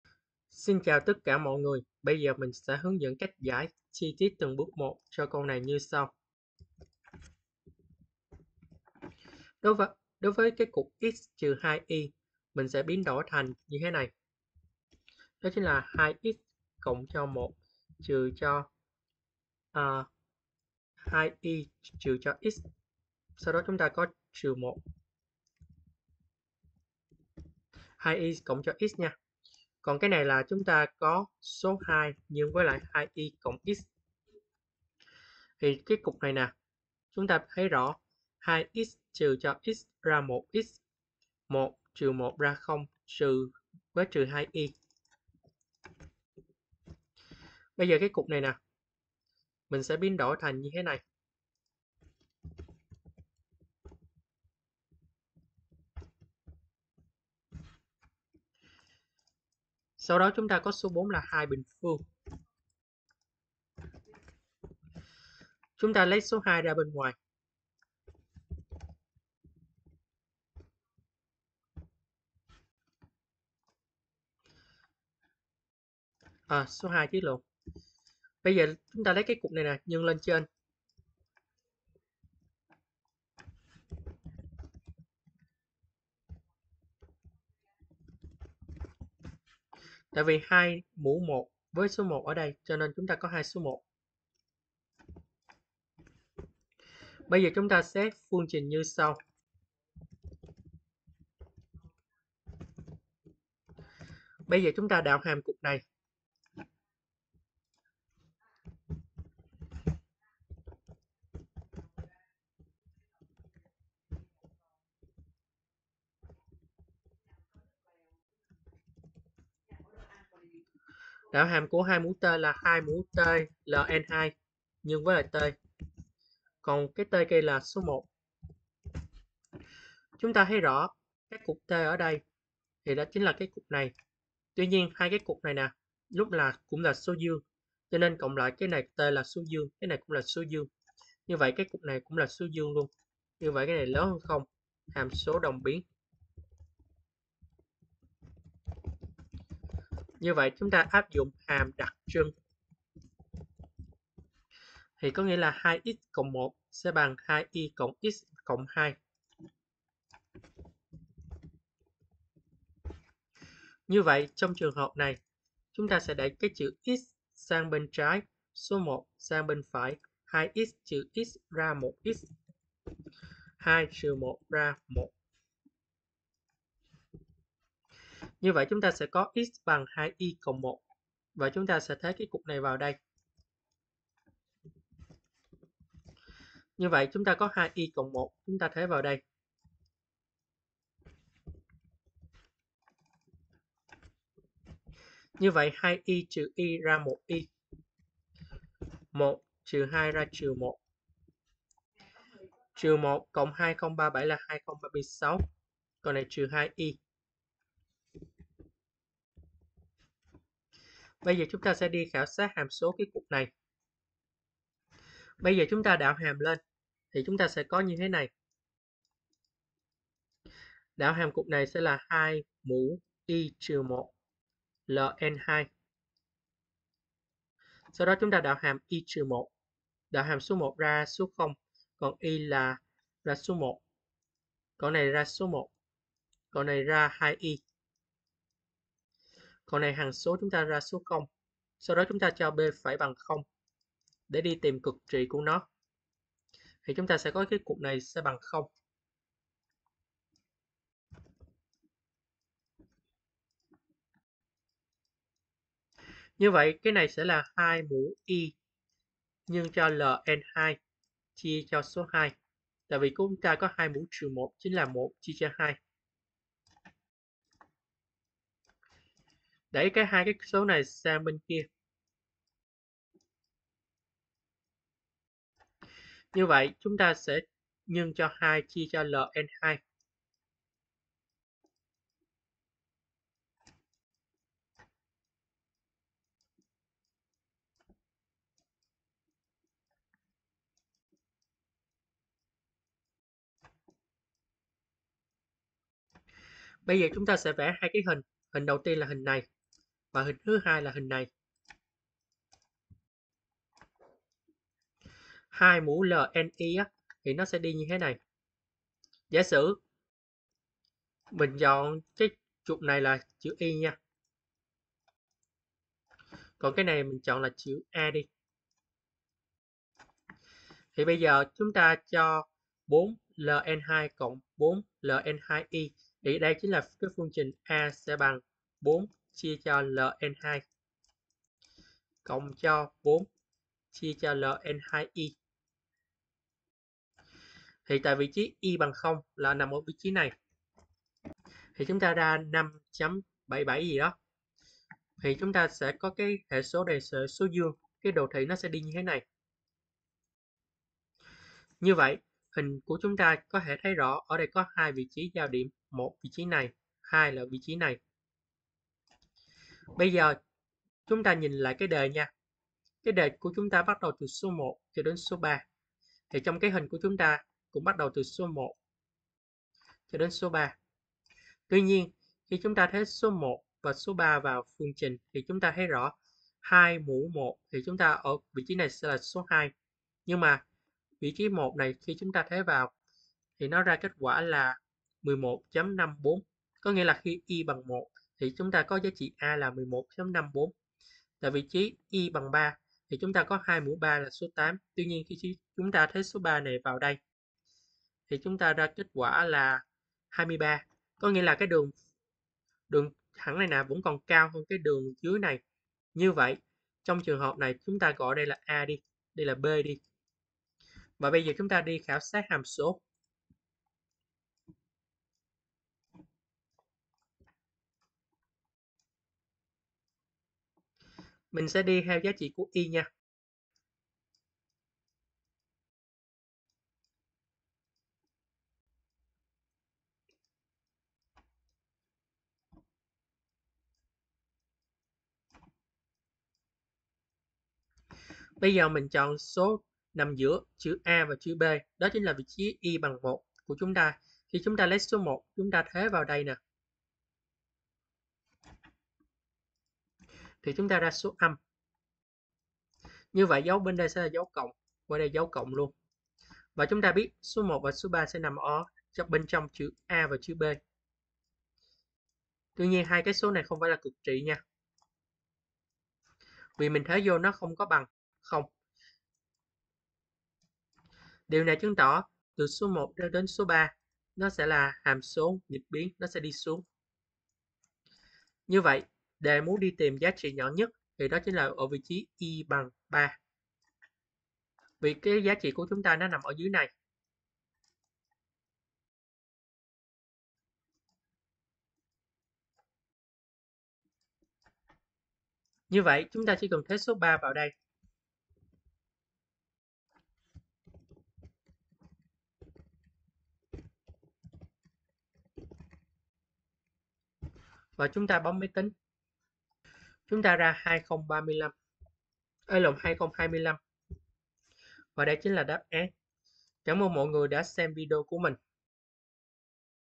Toán 12: Hàm đặc trưng: Cho hai số thực x≥0;1≤y≤3 thỏa mãn 2^(x-2y) (2x+1)=4y+2x+4. Tìm giá trị
Cho hai  số thực x≥0;1≤y≤3 thỏa mãn 2^(x-2y)  (2x+1)=4y+2x+4
Tìm giá trị nhỏ nhất biểu thức P=2^(x-y-2)-x-y^2+2037?